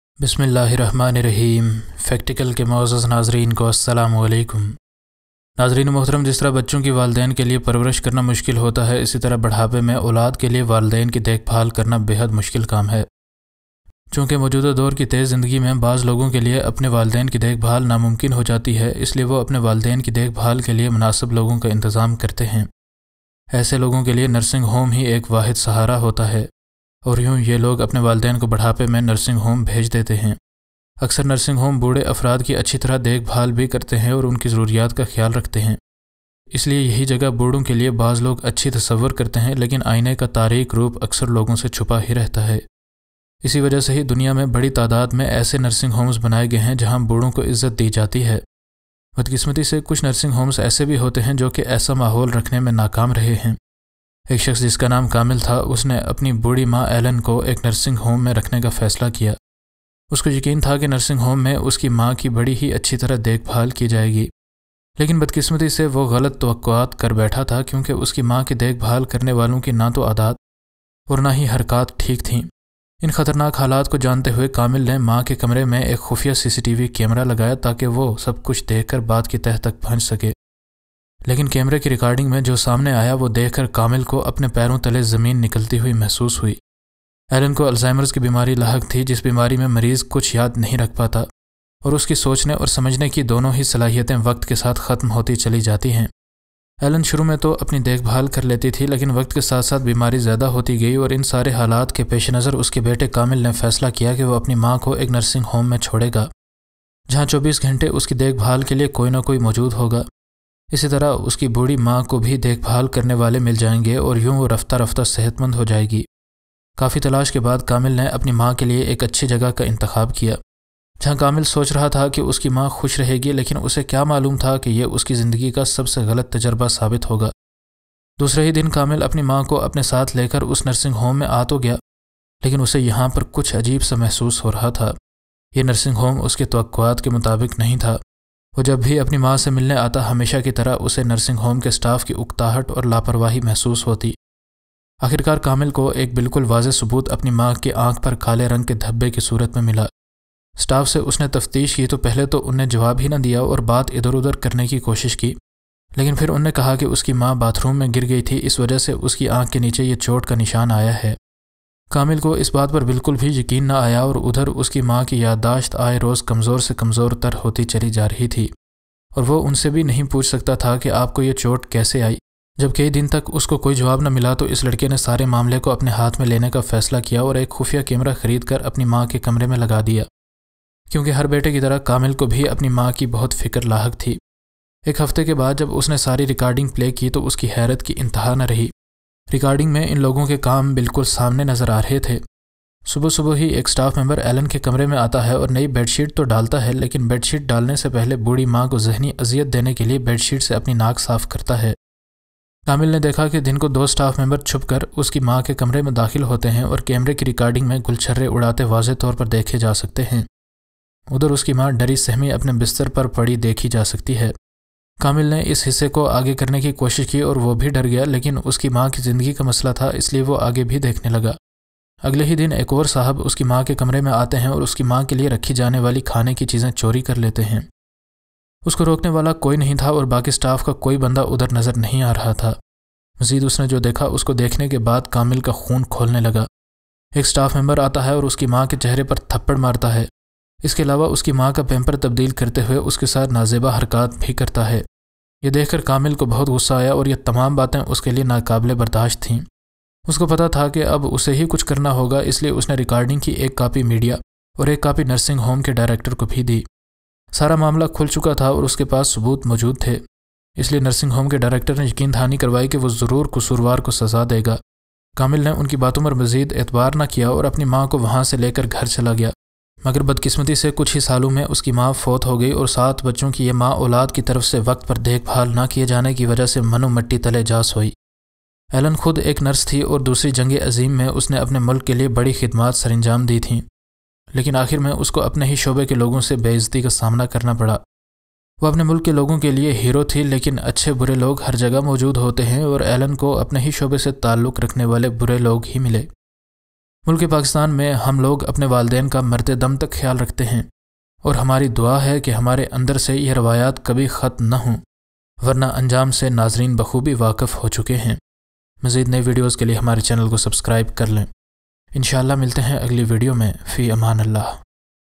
Man Sets Up Hidden Camera To Find Out The Reality of Nursing Home In Which He Admitted His Mother
This man sets up a hidden camera inside the room of her mother to see what the reality of the nursing home is. The man always thought that the attendants of the nursing home were lazy but later he came to realize what they were doing.

For Brand Promotion and other Queries